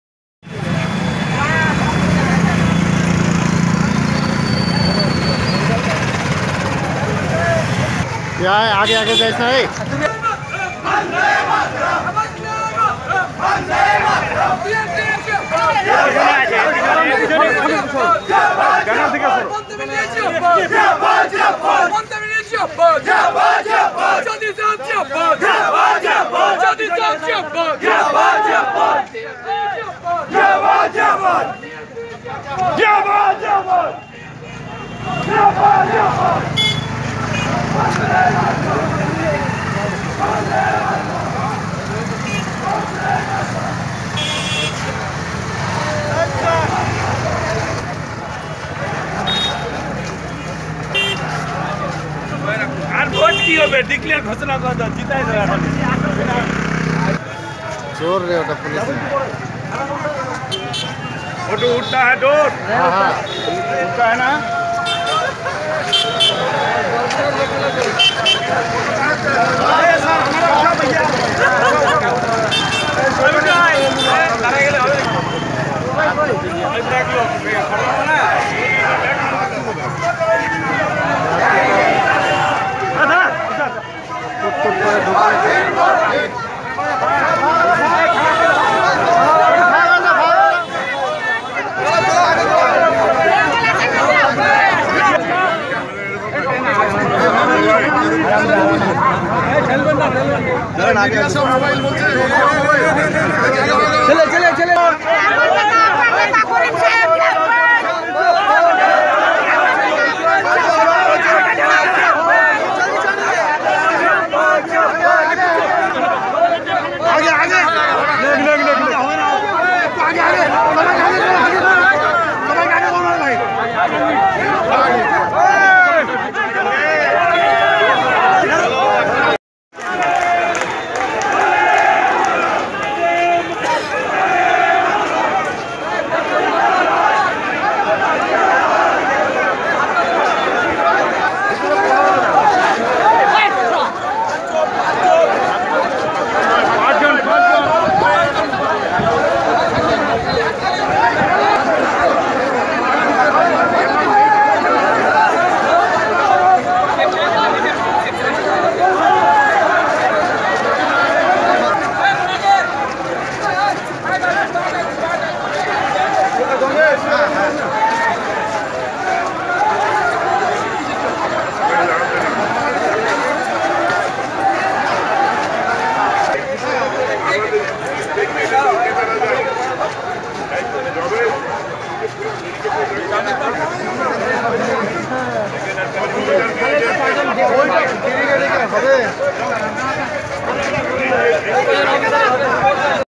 মনোনয়নপত্র জমা দিলেন তৃণমূল নেতা আব্দুল করিম চৌধুরী
ইসলামপুর বিধানসভার উপনির্বাচনে মনোনয়নপত্র জমা দিলেন তৃণমূল নেতা আব্দুল করিম চৌধুরী